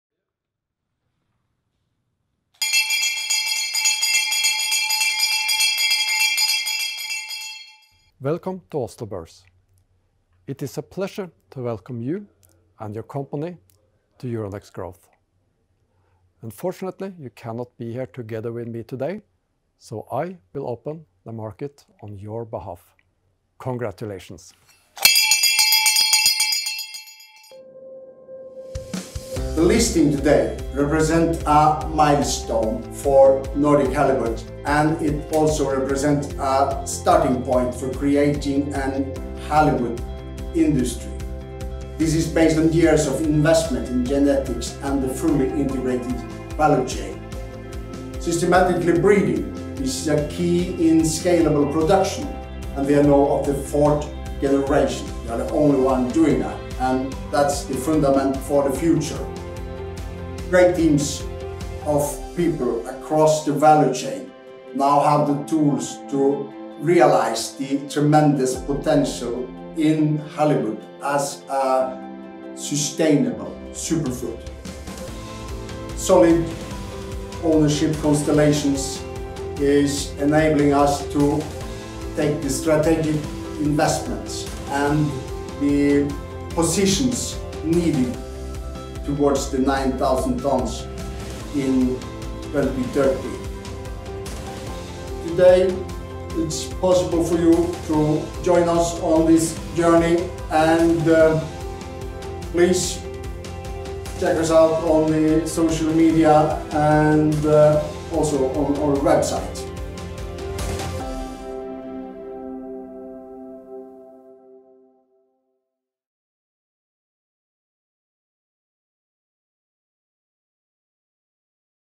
Nordic Halibut - Euronext Growth
Nordic Halibut listed on Euronext Growth Oslo 26 April 2021 | Nordic Halibut notert på  Euronext Growth Oslo 26. april 2021